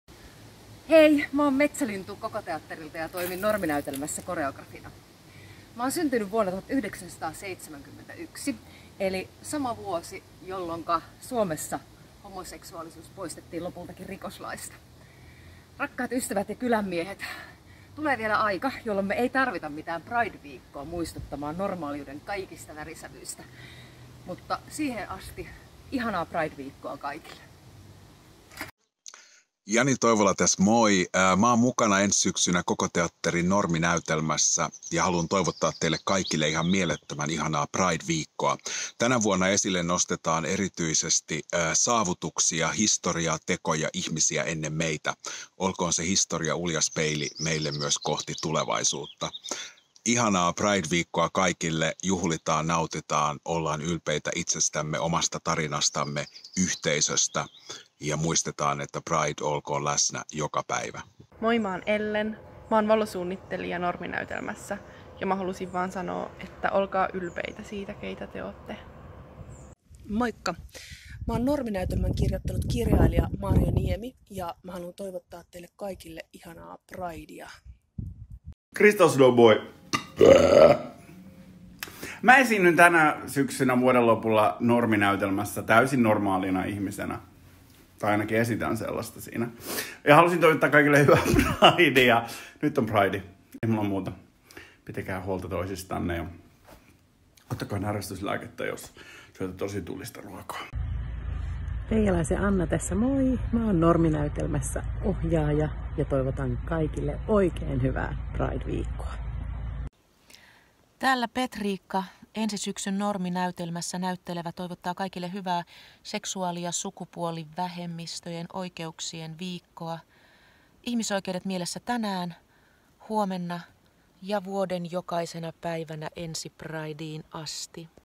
Norminäytelmän työryhmä toivottaa hyvää Helsinki Pridea 2021
Helsinki Pride Weekin kunniaksi kuulemme Norminäytelmän työryhmäläisten videotervehdyksiä. Ihmisoikeudet ovat meillä mielessä tänään, huomenna ja vuoden jokaisena päivänä.

Normeihin on turvallista maastoutua. Mutta entä jos ei pysty? Norminäytelmässä käsitellään sukupuolen ja seksuaalisuuden normeja. Esityksen näyttelijät; Cécile Orblin, Petriikka Pohjanheimo, Cristal Snow sekä Jani Toivola, tekevät siinä neljäkymmentä roolia. Lue lisää esityksestä

KokoTeatteri ja Norminäytelmä toivottavat kaikille hyvää Helsinki Pride Weekia 2021!